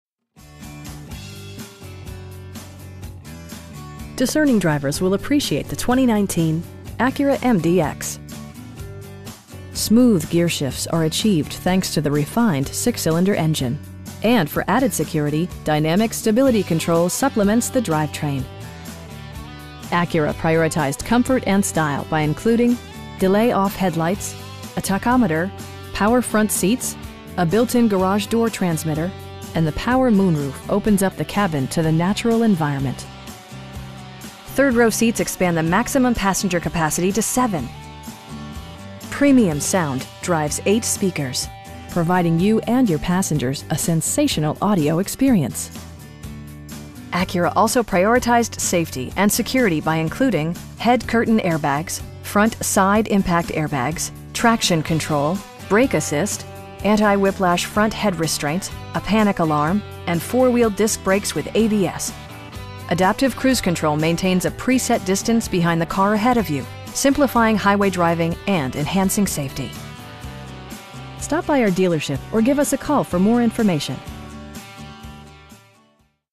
2019 Acura MDX Base in Pleasanton, CA 94588
Discerning drivers will appreciate the 2019 Acura MDX. Smooth gearshifts are achieved thanks to the refined 6 cylinder engine, and for added security, dynamic Stability Control supplements the drivetrain. Acura prioritized comfort and style by including: delay-off headlights, a tachometer, power front seats, a built-in garage door transmitter, power windows, an overhead console, and the power moon roof opens up the cabin to the natural environment. Third row seats expand the maximum passenger capacity to 7. Premium sound drives 8 speakers, providing you and your passengers a sensational audio experience. Acura also prioritized safety and security by including: head curtain airbags, front side impact airbags, traction control, brake assist, anti-whiplash front head restraints, a panic alarm, and 4 wheel disc brakes with ABS. Adaptive cruise control maintains a pre set distance behind the car ahead of you, simplifying highway driving and enhancing safety. We pride ourselves on providing excellent customer service. Stop by our dealership or give us a call for more information.